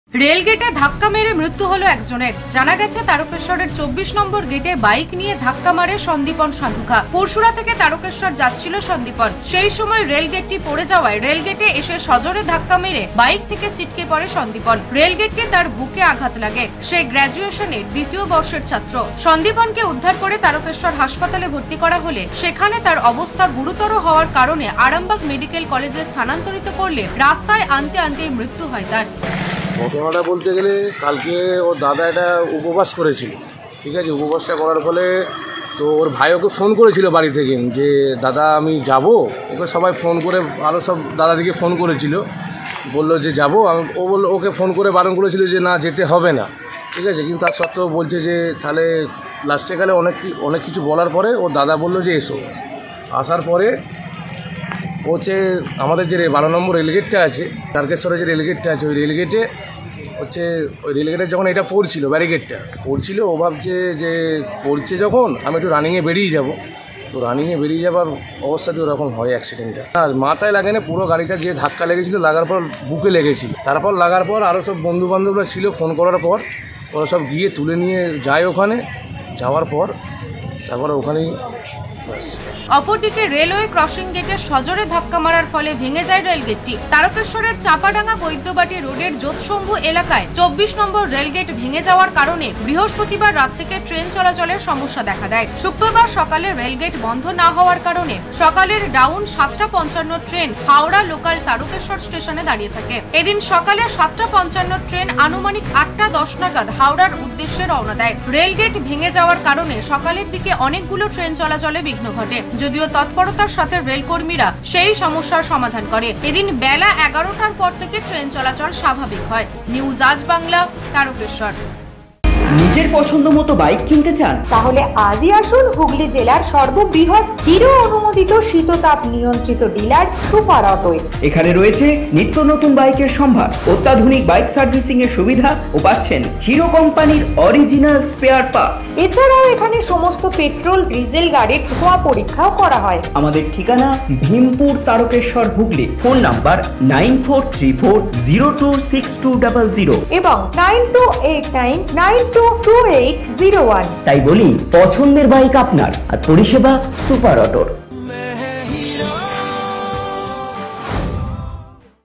রেলগেটে ধাক্কা মেরে মৃত্যু হল ১ যুবকের
রেলগেটে ধাক্কা মেরে বাইক থেকে ছিটকে পরে মৃত্যু হল কলেজ ছাত্রের